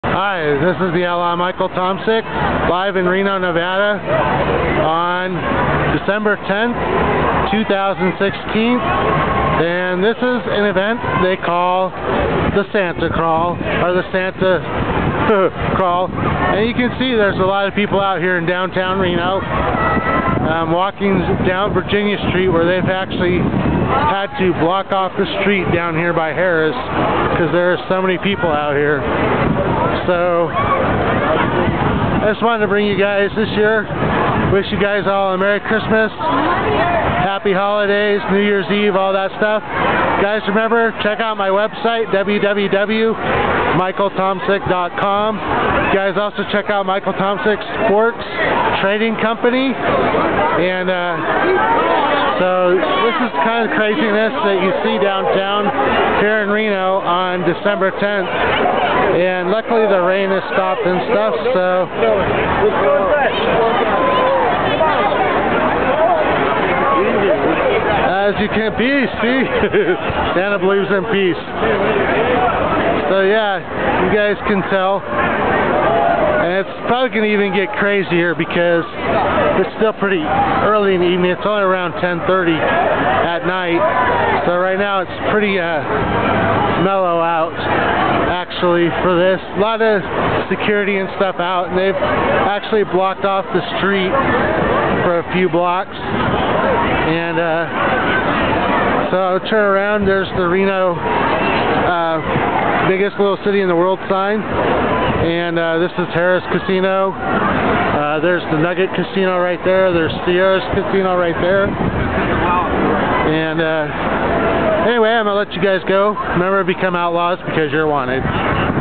SANTA CRAWL RENO NV 89439
Santa Crawl 2016 live from Reno NV